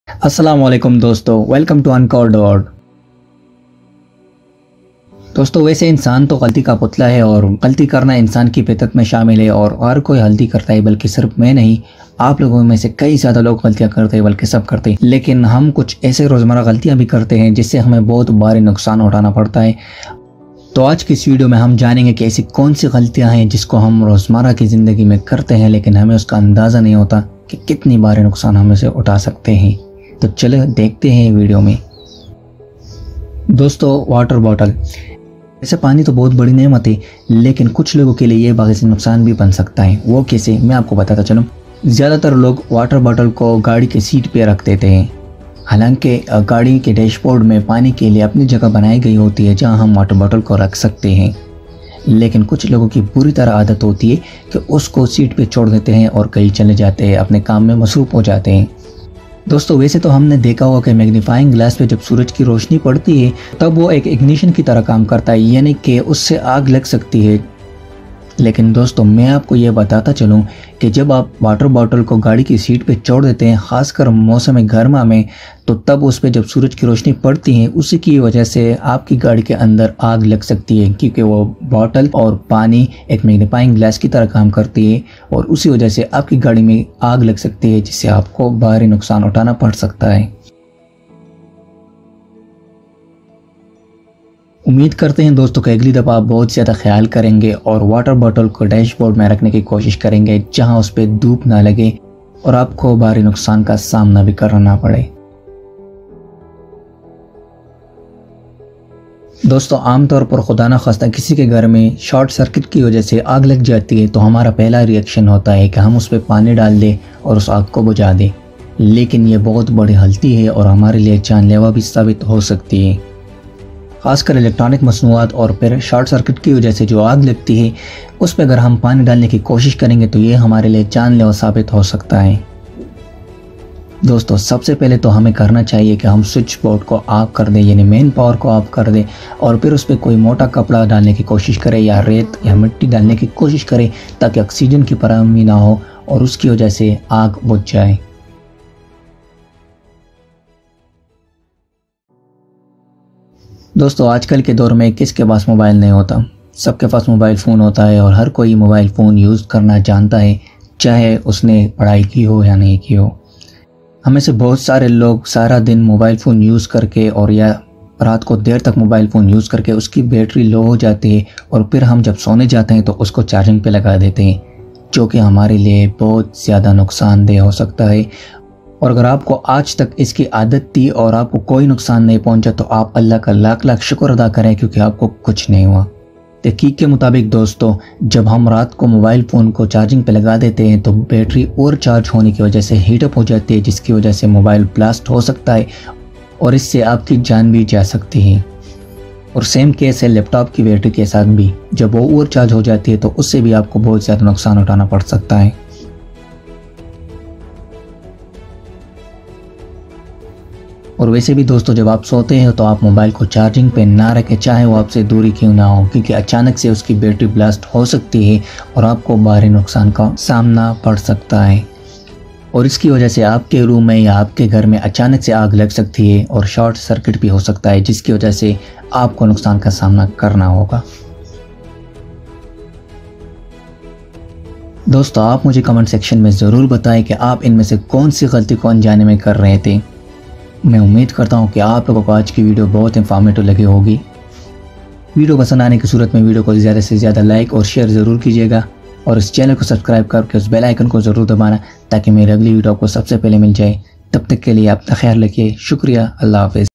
Daily Life Mistakes That Can Cost You More Then You Think | Urdu & Hindi | Uncover The Tech
Daily mistake which can cost us more than our thoughts.We really do not know about these mistakes which often we are doing we should need to know about these mistakes.